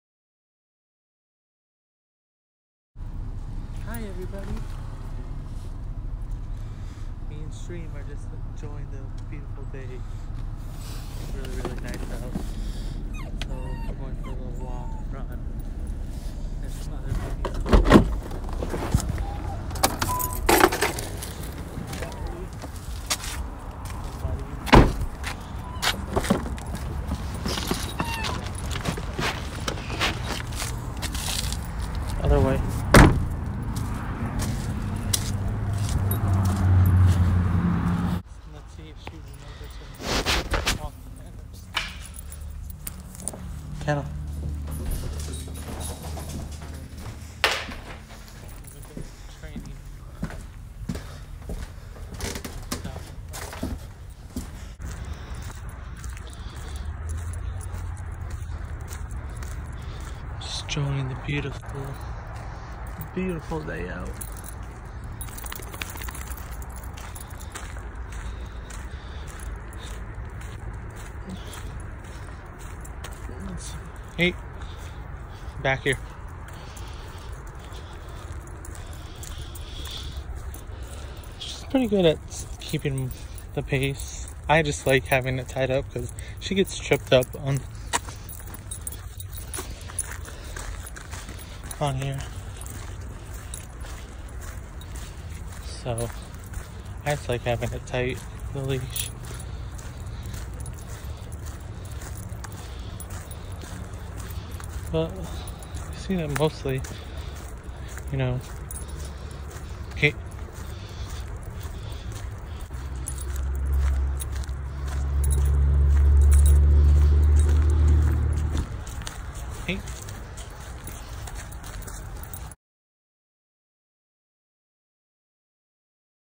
Wire Hair dachshund going for a walk.
Stream is going for a walk on this beautiful day.